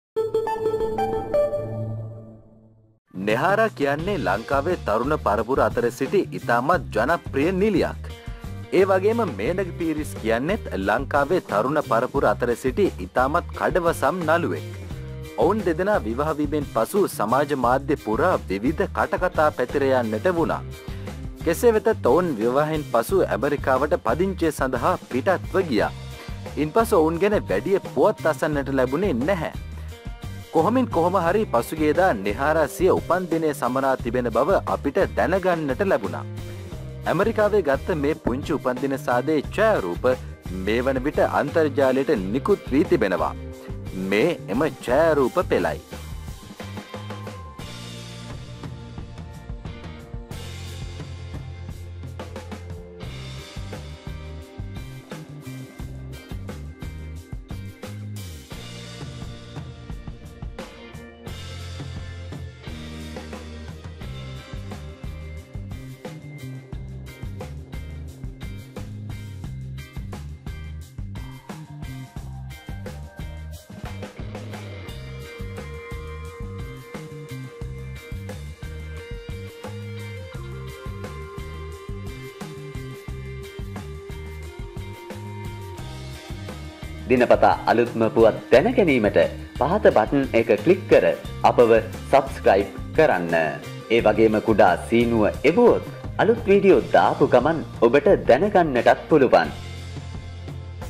නෙහාරා පීරිස් මේනක සමඟ ඇමරිකාවේ සිට උපන්දිනය සැමරූ හැටි මෙන්න
නෙහාරා කියන්නේ ලංකාවේ තරුණ පරපු අතර සිටි ඉතාමත් ජනප්‍රිය නිලියක්. ඒ වගේම මේනක පිරිස් කියන්නෙත් ලංකාවේ තරුණ පරපුර අතර සිටි ඉතාමත් කඩවසම් නළුවෙක්. ඔවුන් දෙදෙනා විවහ වීමෙන් පසු සමාජ මාධ්‍ය පුරා විවිධ කටකතා පැතිරයන්නට වුණා.

කෙසේ වෙතත් ඔවුන් විවාහයෙන් පසු ඇමරිකාවට පදිංචිය සඳහා පිටත්ව ගියා. ඉන්පසු ඔවුන් ගැන වැඩිය පුවත් අසන්නට ලැබුනේ නැහැ. කොහොමින් කොහොම හරි පසුගියදා නෙහාර සිය උපන්දිනය සමරා තිබෙන බව අපිට දැනගන්නට ලැබුනා. ඇමරිකාවේ ගත්ත මේ පුංචි උපන්දින සාදයේ ඡායරූප මේ වනවිට අන්තර්ජාලයට නිකුත්වී තිබෙනවා මේ එම ඡායරූප පෙළයි